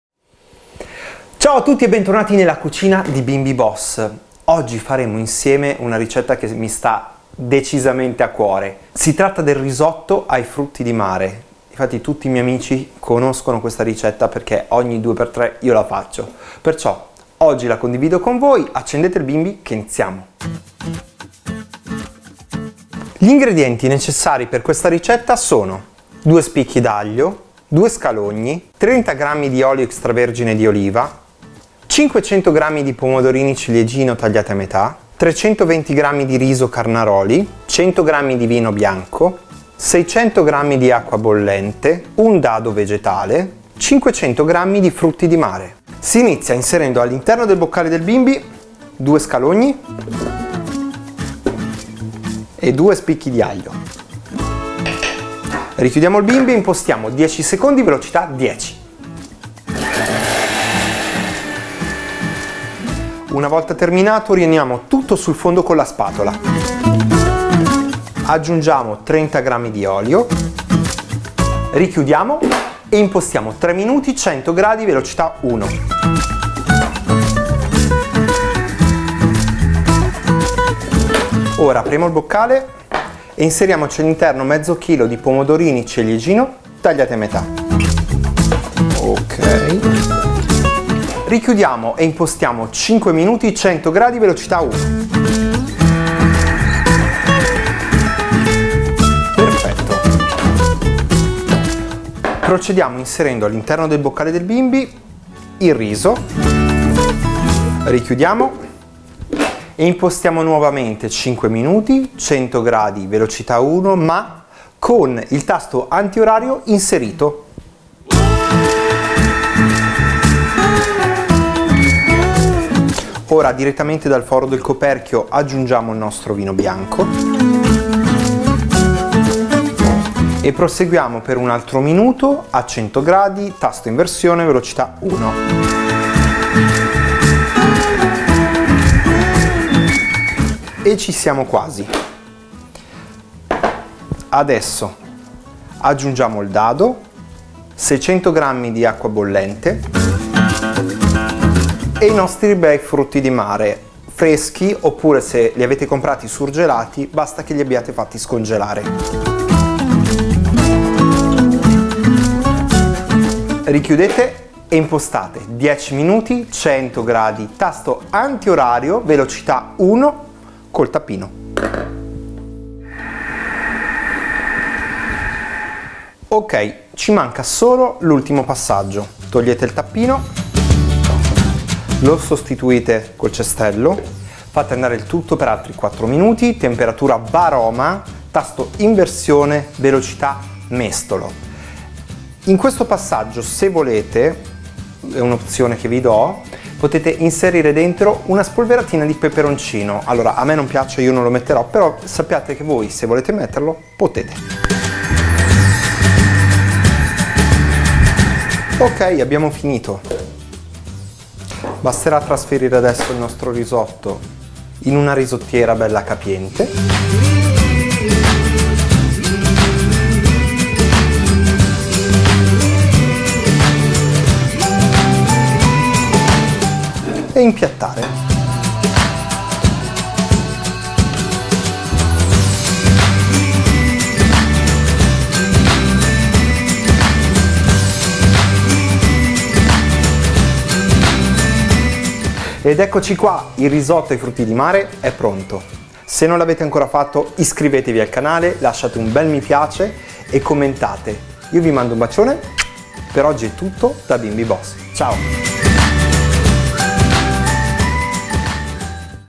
Risotto ai frutti di mare | Bimby TM6 TM5 -TM31 | Thermomix
ll risotto ai frutti di mare è un primo piatto classico della cucina italiana, ottimo sia nei mesi freddi che in quelli caldi. Ovviamente il risotto ai frutti di mare ha un risultato strepitoso se preparato con del buon pesce fresco, anche se la sua preparazione ovviamente sarà economicamente più dispendiosa.
Anche se in apparenza  può sembrare una ricetta assai complessa, in realtà il risotto ai frutti di mare non richiede particolare perizia e abilità tra i fornelli, l’importante è scegliere accuratamente il pesce con cui preparare questa pietanza e dotarsi di un po' di pazienza.

Il risotto ai frutti di mare che vi proponiamo in questa ricetta è preparato utilizzando cozze, vongole, gamberi e calamari ma questa tipologia di pesce  può variare a seconda dei propri gusti o in base alla disponibilità del pesce.